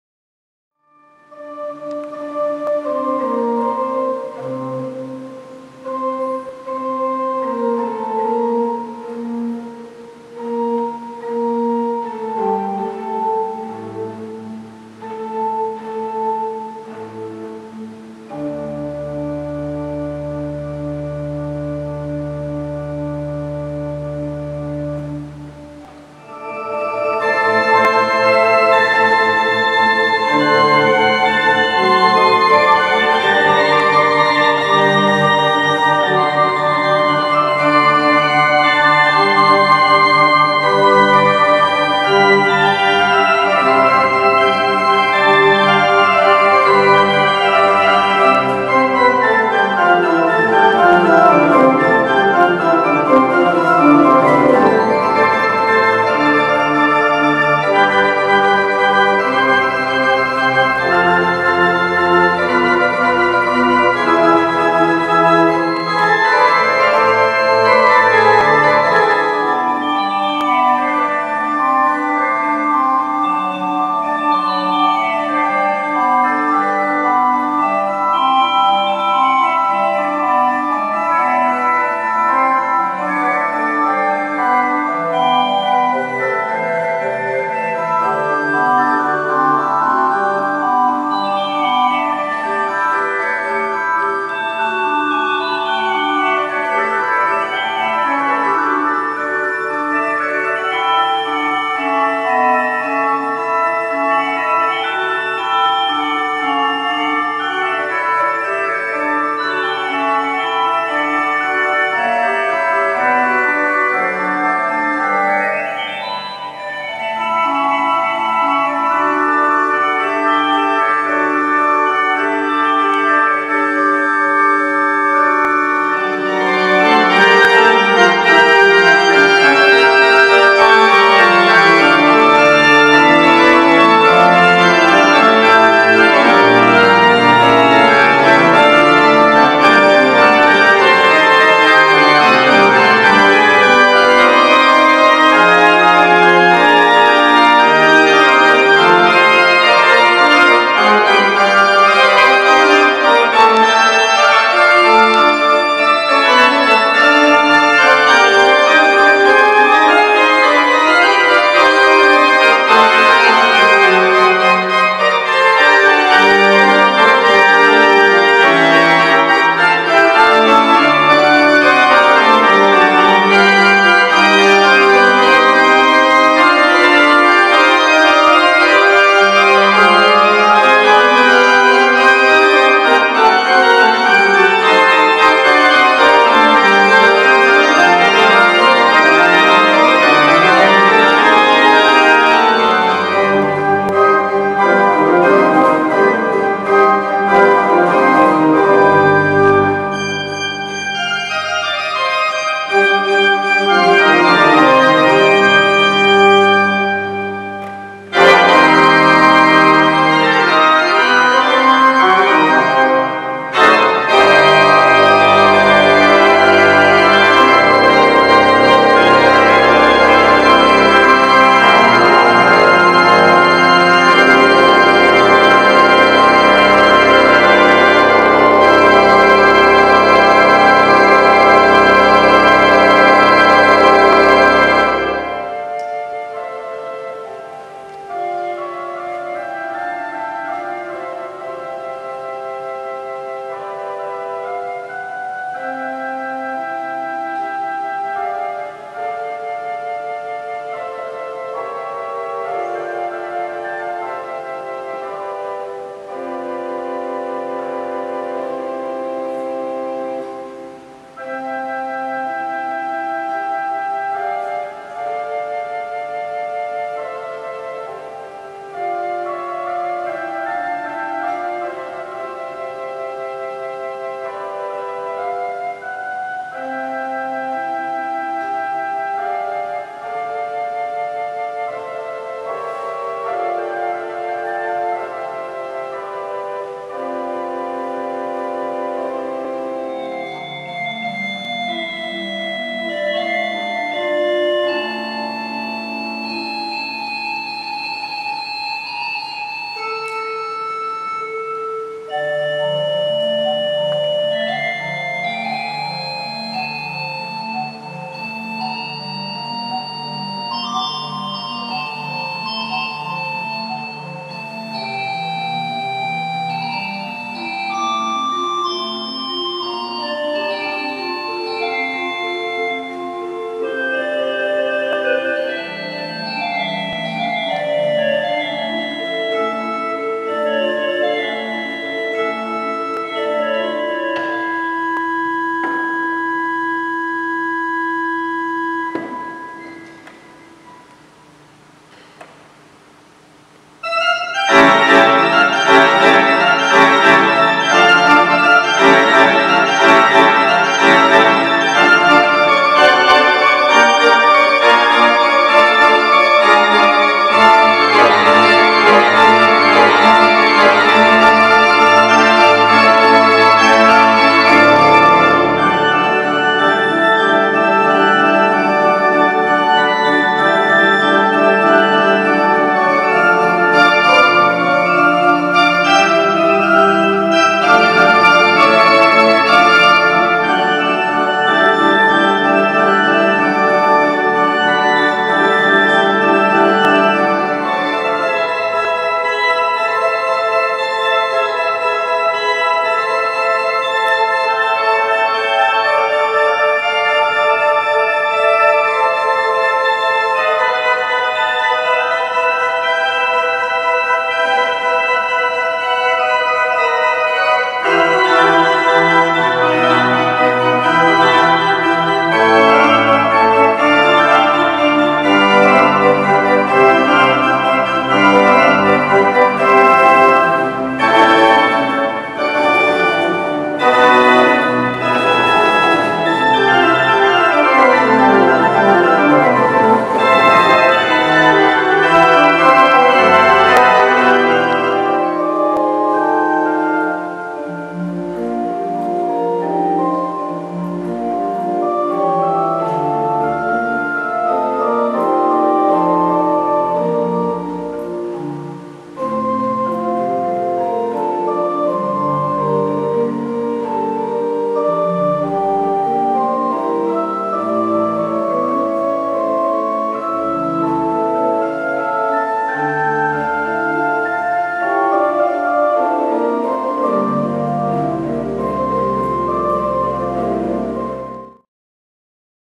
Koncert organowy Alessandro Canale | skarzysko.info
W środowy wieczór w skarżyskiej parafii pw. św. Brata Alberta odbył się koncert muzyki organowej. Najbardziej znane utwory zagrał dla skarżyszczan włoski organista, Alessandro Canale.